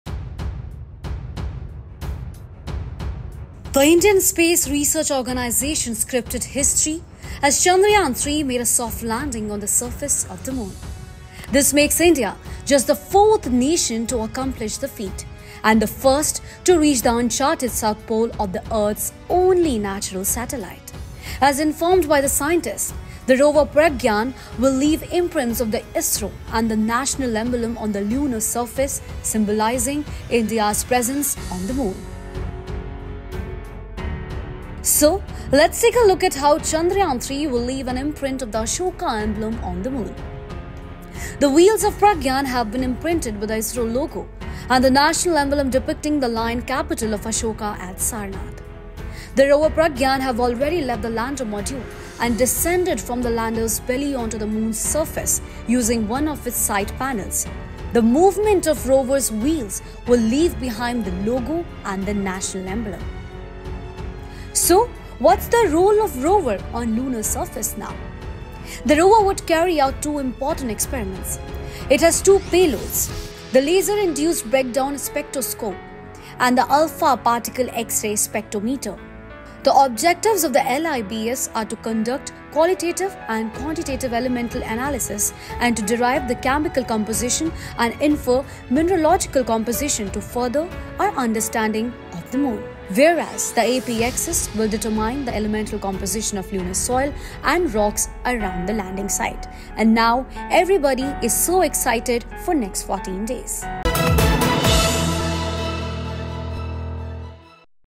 Chandrayaan-3: How Pragyan Rover Will Engrave India's National Emblem on the Moon
The Indian Space Research Organisation (ISRO)'s Chandrayaan-3 mission has made a soft landing on the moon, and the Pragyan rover is now set to explore the lunar surface. The rover will carry a special device that will engrave the national emblem of India on the moon, symbolizing India's presence on the lunar surface. Watch this video to learn more about how Pragyan will engrave the national emblem on the moon.